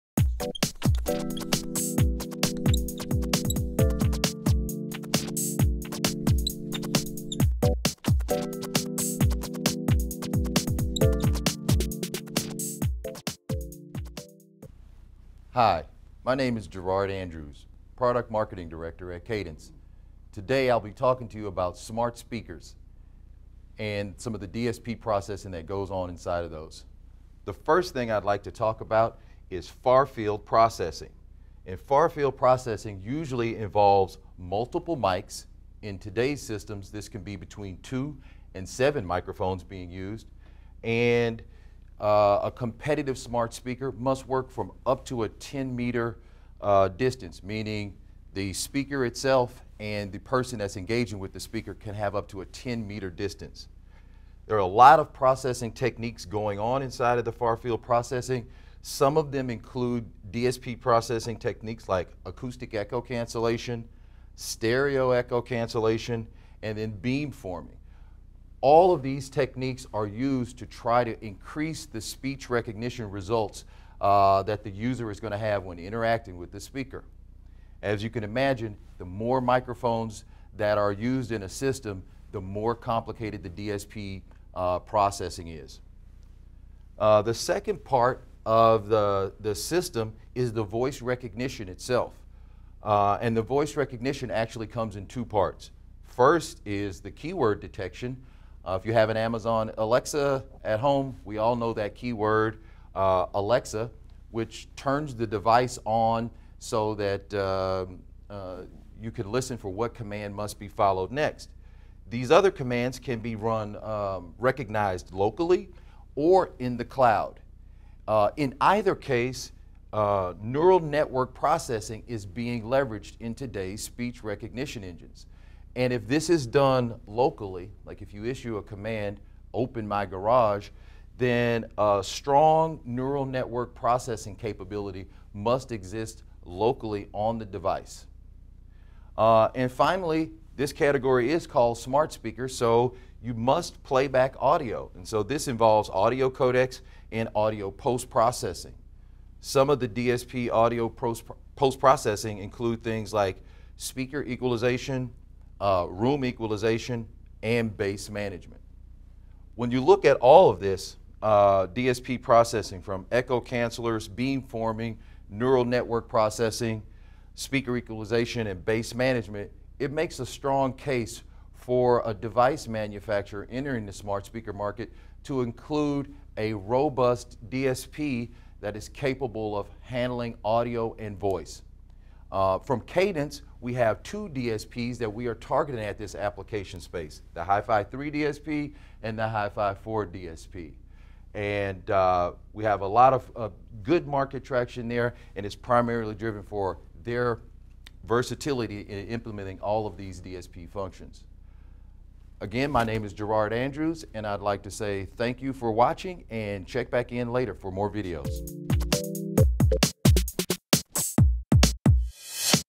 Whiteboard Wednesdays - Smart Speakers and Audio DSP Processing
In this week’s Whiteboard Wednesdays video Gerard Andrews discusses the three areas of smart speakers that require high-performance audio DSPs to process these features.